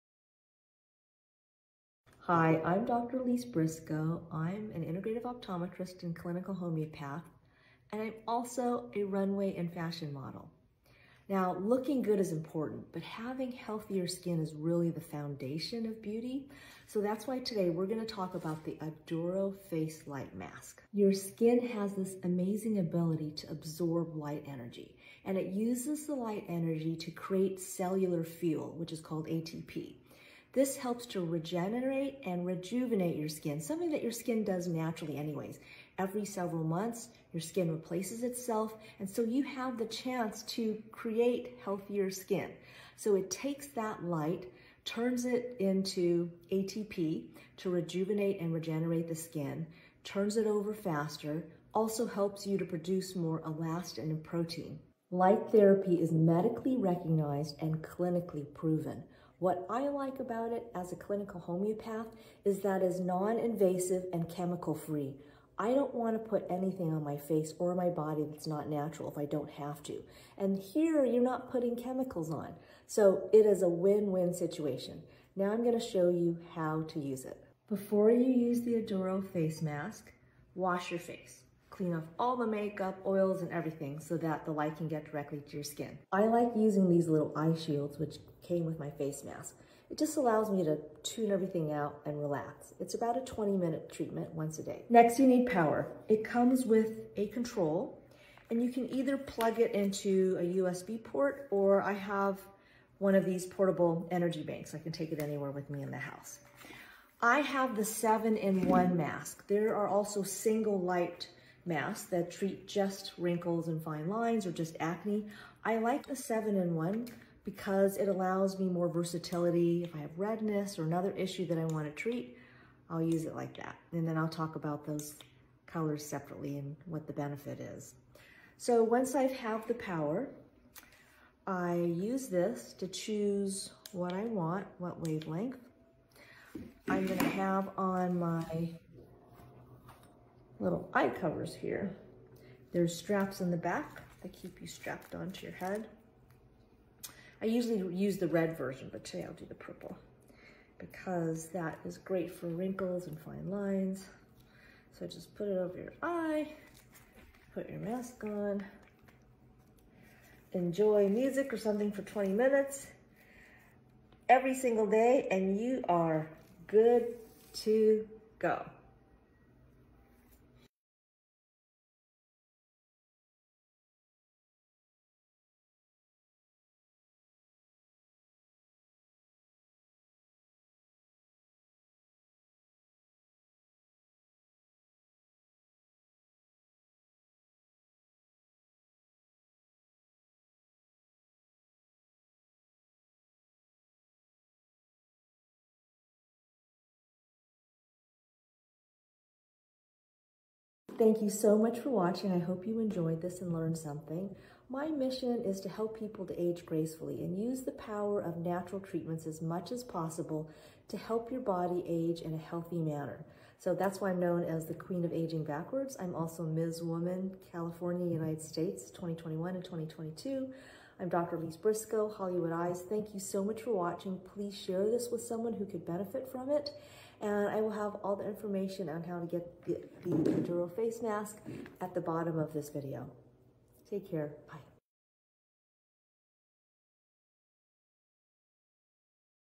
Unlocking Flawless Skin: Expert Endorsement of the Aduro Light Therapy Mask
Hi everyone! As an innovative ophthalmologist and clinical homeopath, as well as a runway and fashion model, it's important to look great, but having healthy skin is the foundation of beauty. So today, I want to share with you the Aduro Light Therapy Mask.

Every few months, your skin cells naturally turn over, which gives you the opportunity to create healthy skin. Your skin has the amazing ability to absorb light energy, which it uses to produce cellular fuel, or ATP, which helps regenerate and restore your skin, which is actually the skin's natural ability to repair itself. Phototherapy converts light energy into ATP, which accelerates the regeneration and renewal of your skin while helping you produce more elasticity and protein. Phototherapy has been recognized and clinically proven by the medical community.

As a clinical professional, I love that light therapy is non-invasive and chemical-free. I don't want to put anything on my face or body that isn't natural, and when using a light therapy mask, you don't need to be exposed to chemicals, which is a win-win situation.

I show you how to use it in the video. Before using the Aduro Light Therapy Mask, wash your face to clean off all makeup and oils so that the light hits your skin directly.

The violet light from the Aduro Light Therapy Mask is very effective for wrinkles and fine lines. Put the eye mask on, put the mask on, enjoy music or something else for 20 minutes, once a day, and you're set.

As the unofficial "Queen of Aging Reversal". I want to help people age gracefully and healthily and utilize the power of natural healing as much as possible.